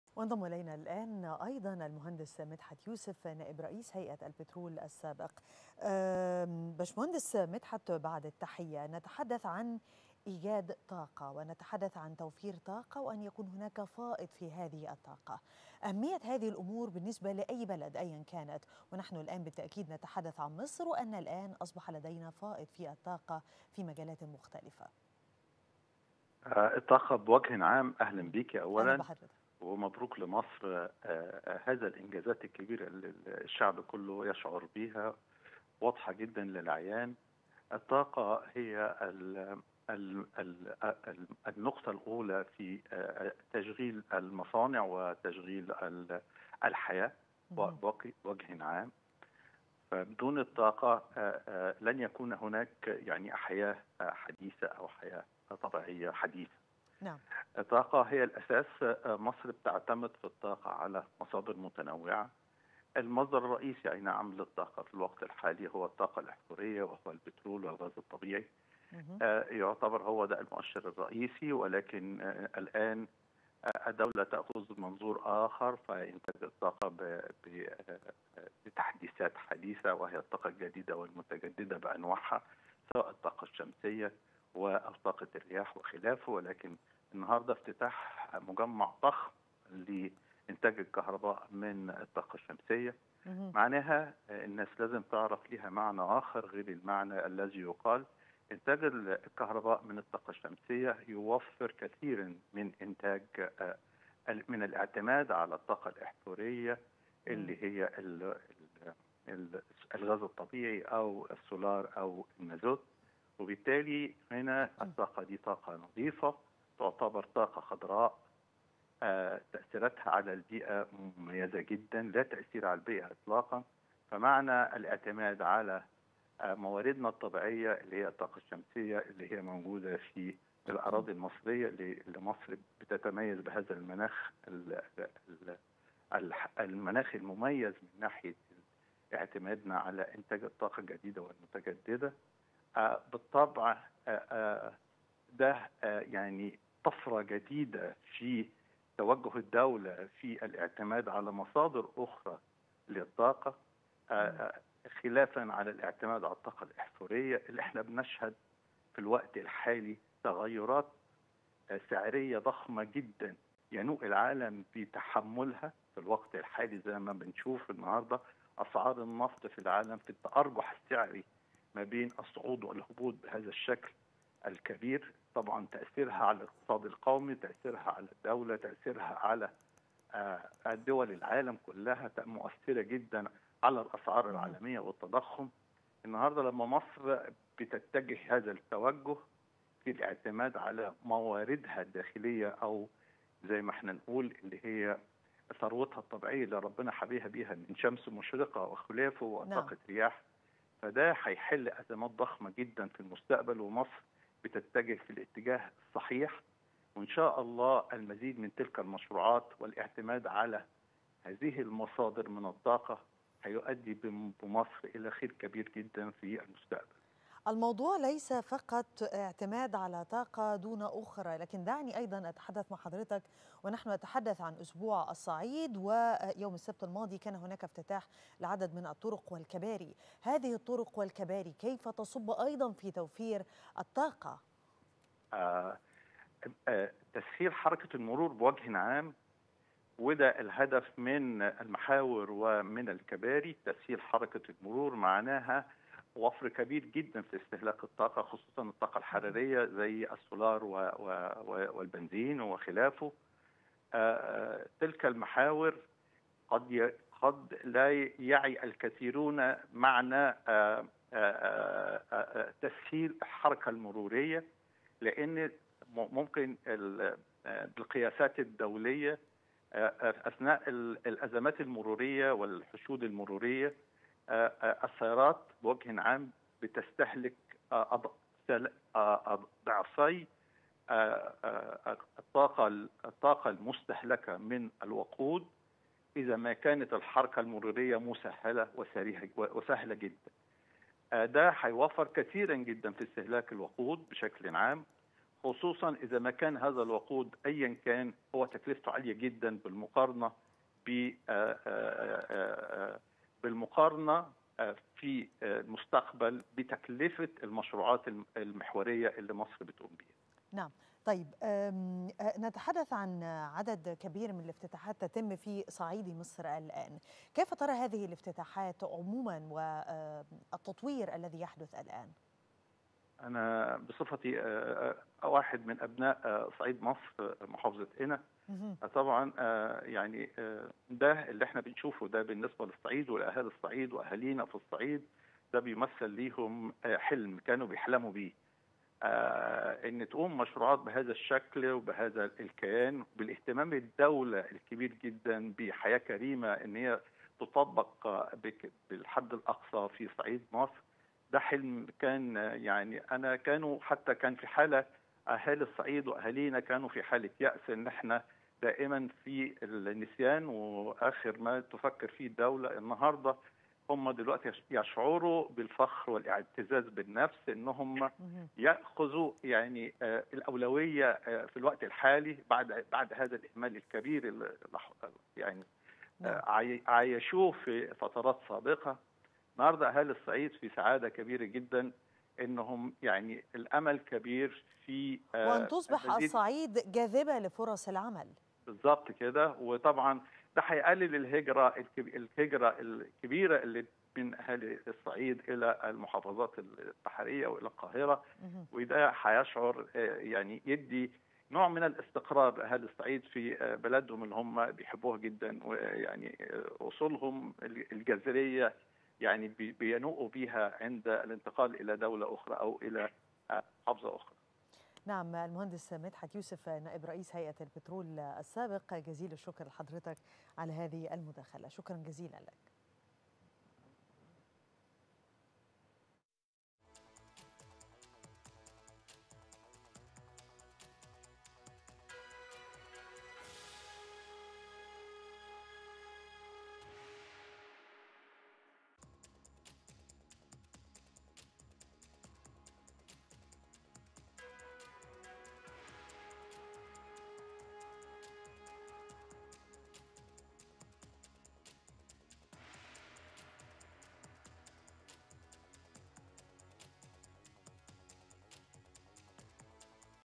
المهندس مدحت يوسف نائب رئيس هيئة البترول سابقا: بدون الطاقة لن تكون هناك حياة حديثة
27  ديسمبر 2021

المهندس مدحت يوسف نائب رئيس هيئة البترول سابقا